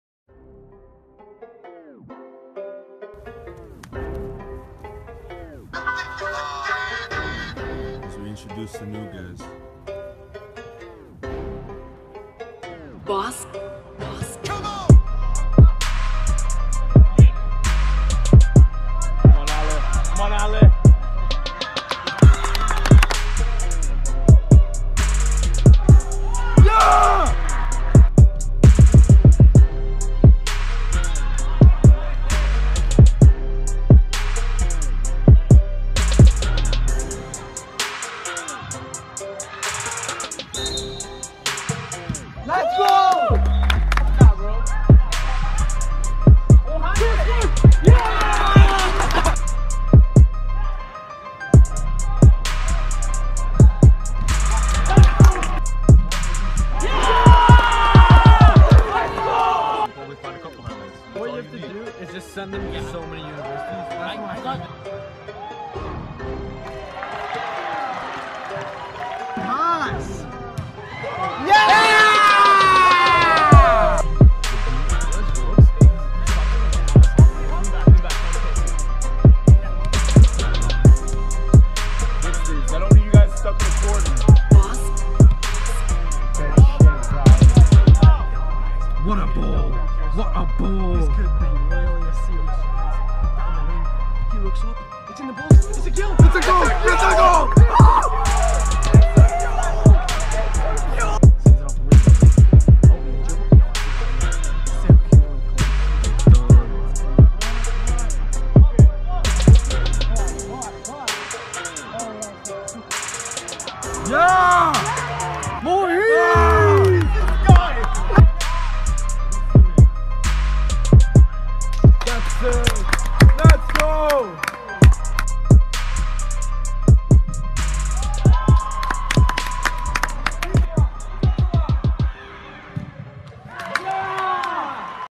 SC Toronto 2000 / 2001 Boys - PISL 2017-2018 | Beat prod. Boss Beats
SC Toronto U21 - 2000 / 2001 Boys

Provincial Indoor Soccer League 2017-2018.

The beat was produced by Boss Beats.